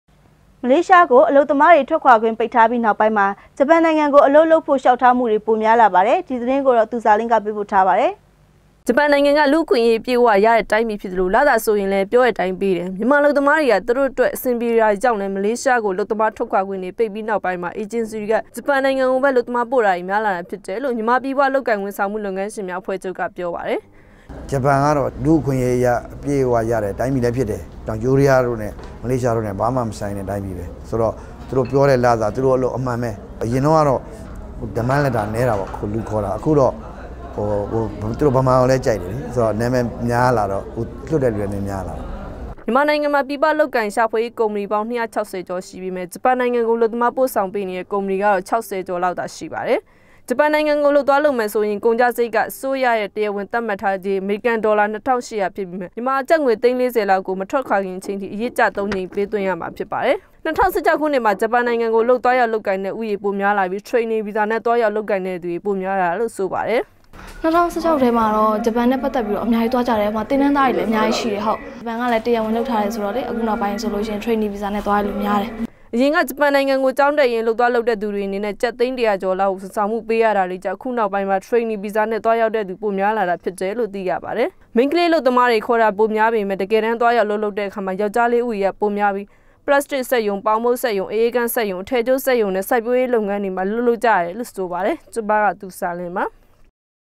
DVB TV - မေလးရွားကို ပိတ္ျပီးထားေနာက္ပုိင္း ဂ်ပန္ႏုိင္ငံကုိ ေလွ်ာက္ထားမႈပုိမ်ား
DVB TV - မေလးရွားကို အလုပ္သမားေတြထြက္ခြာခြင့္ ပိတ္ထားၿပီးေနာက္ပိုင္းမွာ    ဂ်ပန္ႏူိ္င္ငံကို အလုပ္လုပ္ဖို႕ ေလွ်ာက္ထားမႈေတြ ပိုမ်ားလာ ပါတယ္။ ဒီသတင္းကို သူဇာလင္းက ေပးပို႕ထားပါတယ္။